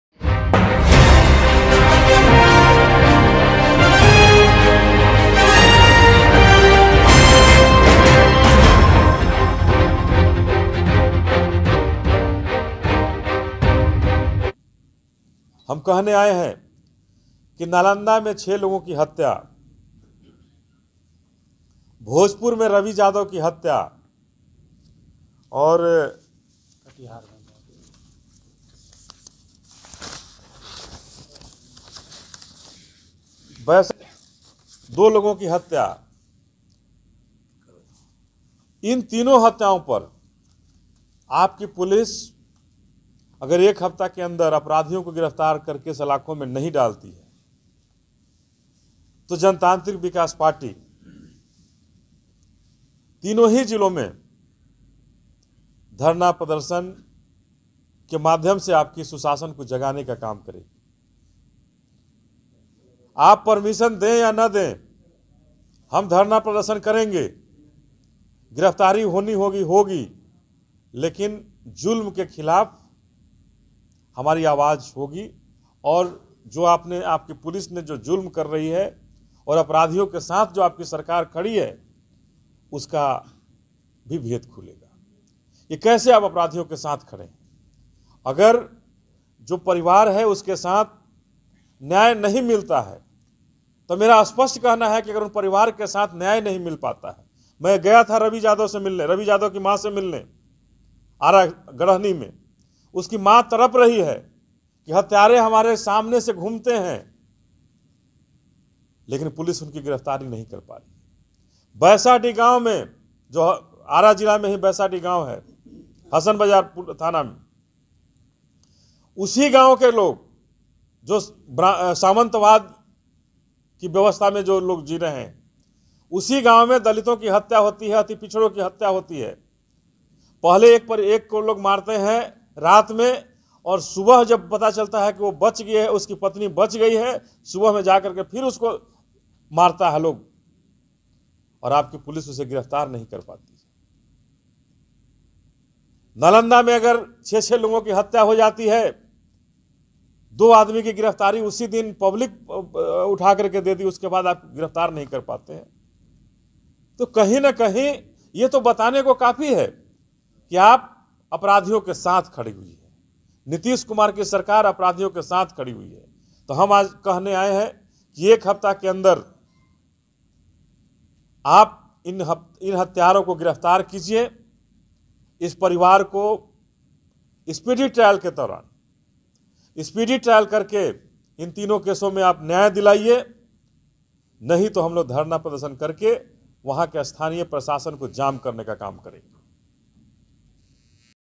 अनिल कुमार ने नितिश कुमार के सुशासन मे बढते अपराध को लेकर पुछा सवाल - ARYAVART TODAY
जनतांत्रिक विकास पार्टी के राष्ट्रिय अध्यक्ष ने बिहार मे बढते अपराध को लेकर प्रेस वार्ता कर
 मुख्‍यमंत्री से पूछा कि आखिर बिहार में कब तक अपराधियों की गिरफ्तारी नहीं होगी और नीतीश कुमार जी आप काम करते रहेंगे? हत्‍यारा कोई भी हो जदयू का सुशासन इन अपराधियों को बचाना क्‍यों चाह रही है ? क्‍यों आपने बिहार के लोगों को अपराधियों की कहर में जलने के लिए छोड़ दिया है ? मुख्‍यमंत्री जी आप तो सुशासन का दावा कर सरकार में आये थे, लेकिन कौन सा सुशासन स्‍थापित कर रहे हैं आप? मुंगेर में 8 साल की बच्‍ची से दुष्‍कर्म, कटिहार में मेयर की हत्‍या जैसे मामलों में आपकी पुलिस का बैरंग खाली है, क्‍योंकि सुशासन में हत्‍यारों को संरक्षण मिल रहा है - ARYAVART TODAY